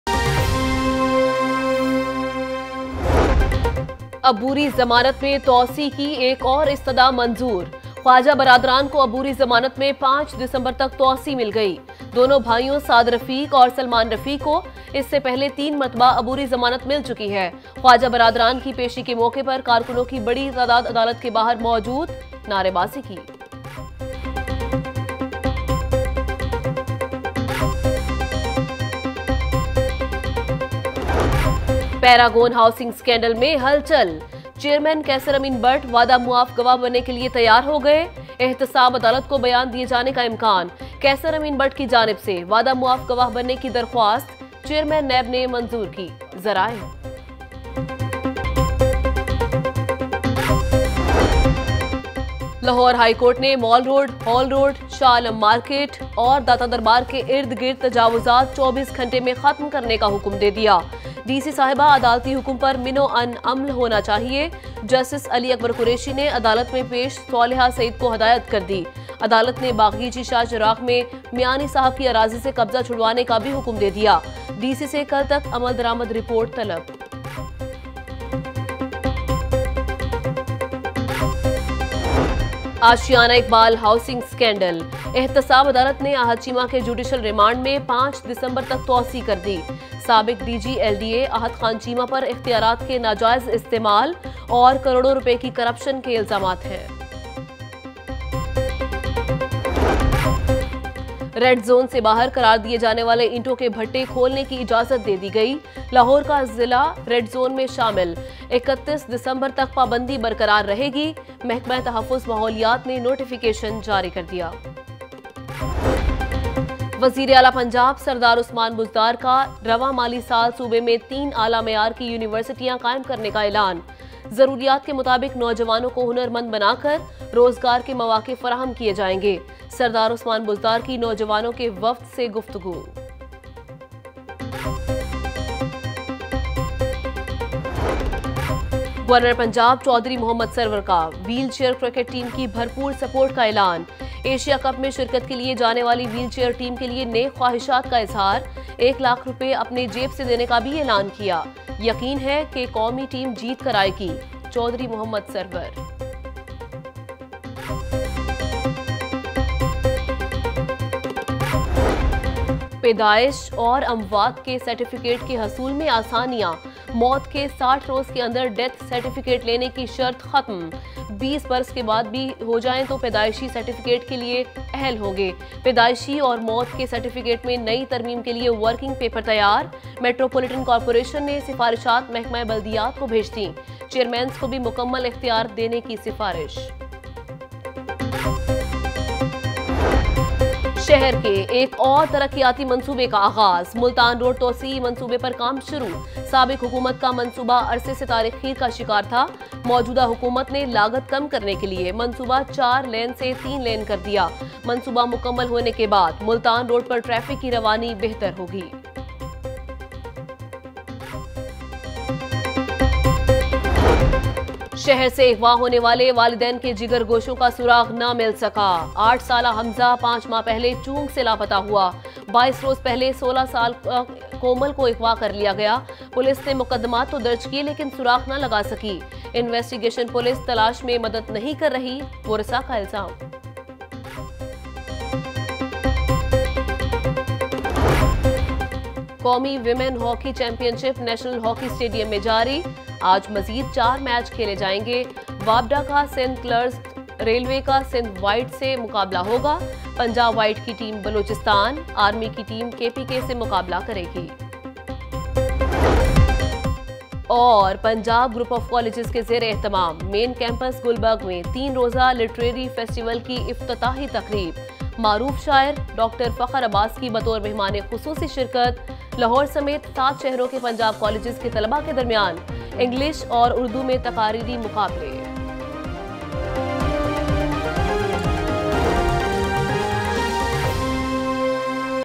01 PM Headlines Lahore News HD – 26th November 2018
Watch Latest News:
Khawaja Saad Rafique bail extended till 5th December in Paragon Housing Scandal, Attack at Chines consulate Karachi, PSCA begins challan on over speeding, NAB Court approves 7-days transit remand of Shehbaz Sharif.  Shehbaz Sharif to appear before NAB Court in Aashiyana Iqbal Scandal, Nation celebrates Eid Milad un Nabi, Roads and streets decorated on Eid Milad un Nabi, CJP orders larger bench  to constitute JIT on Model Town tragedyPTI wins two senate seats, 6 Dead in truck accident near Sundar, Pakistani actor Asad Malik apprehended for carrying weapons at Lahore airport, Nawaz Sharif and Khaqan Abbasi to appear before LHC in treason case, IG Punjab assures to de-politicize police, Lahore among worst polluted cities ,Gas shortage prevails in Lahore, , Gas load shedding starts in different areas across city, Petrol prices witness sharp increase, Dolphin Force officer opens fire at local, Poultry prices witness sharp increase, Dry Fruit prices increases, , Immense traffic jam in Lahore, Scuffle breaks out between locals and traffic wardens, Dollar prices witness sharp increase, Oil tanker collides at Waris Meer underpass and much more.